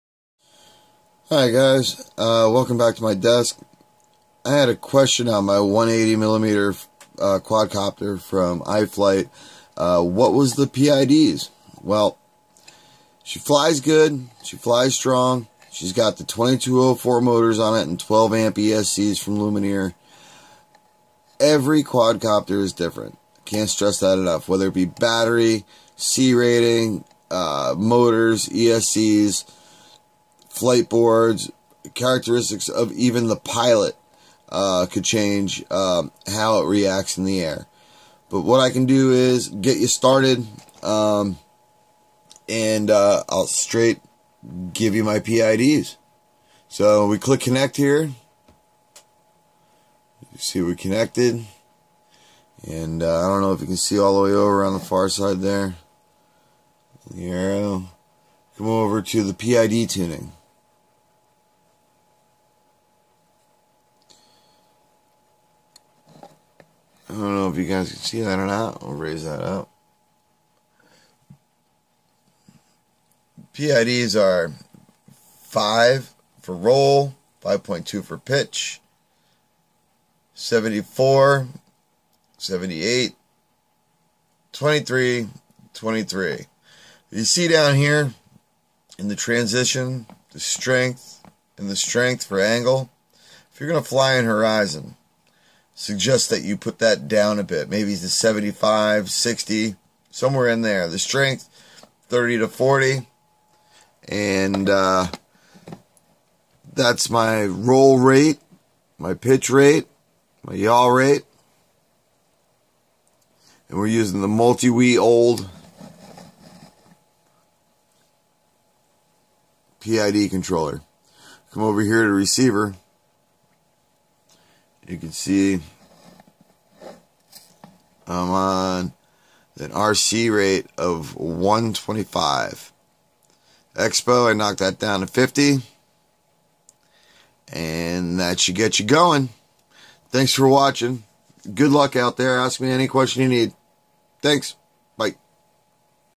PID tuning for zmr180 IFlight
IFlight rc 180mm zmr p.i.d. tuning. Ex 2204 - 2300 kV naze 32 lumenier 12a 1350mah 3s
Every model is different whether it be by weight size or pilot.. I hope these numbers get you started in the right direction it's a little trickier to tune then a 250 or a larger quad. hope you enjoy the video. hope this helps you out.